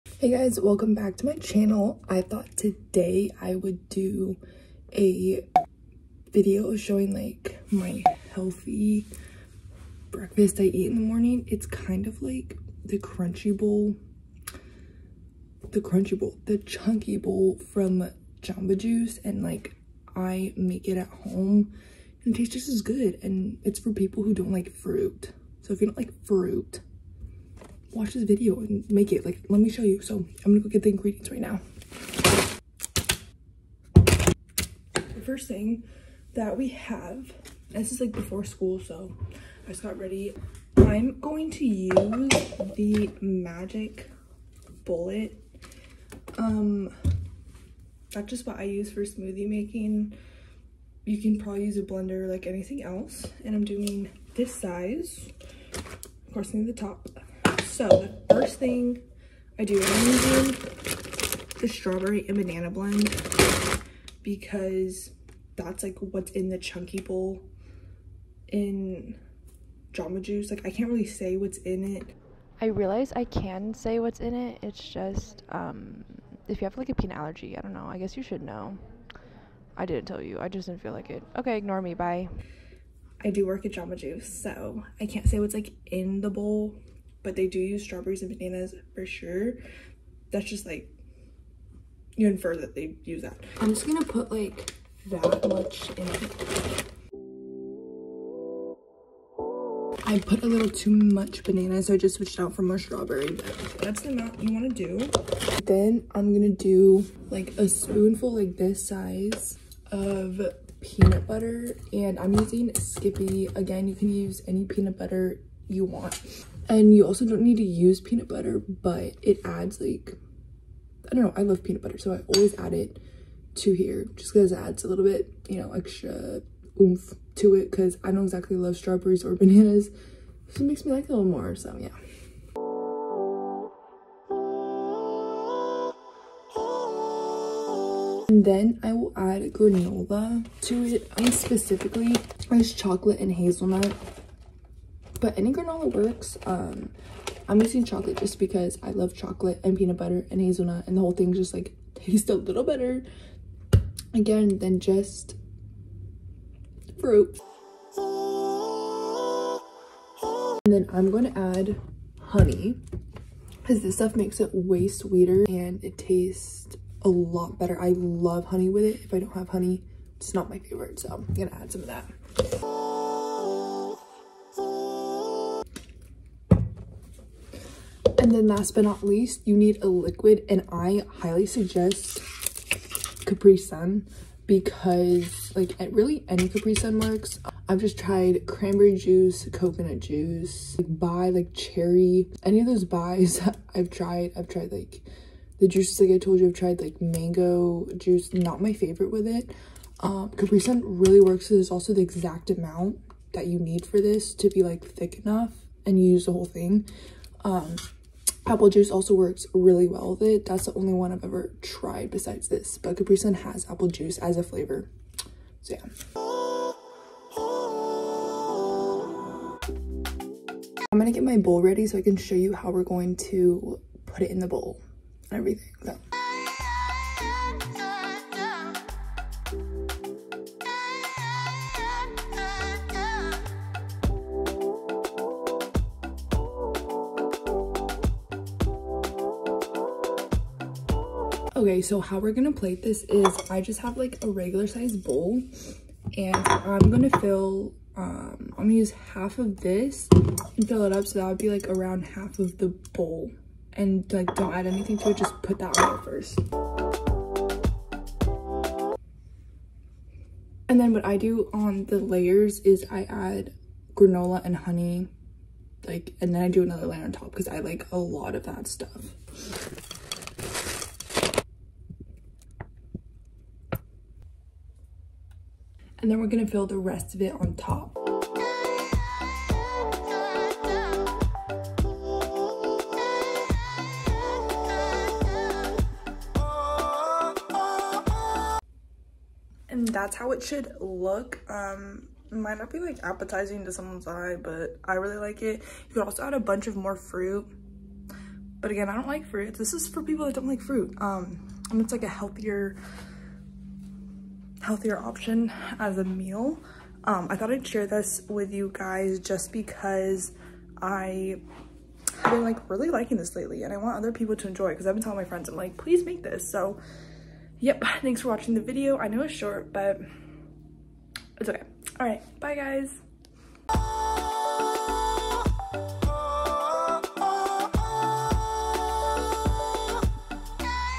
Smoothie Bowl for Picky People | IsabellesDream
This morning I made a strawberry bowl based off of Jamba Juice's Chunky Strawberry bowl. I hate fruit so I made a bowl that is some-what healthy for you that is acceptable for people like me. Kinda messy video but here we are!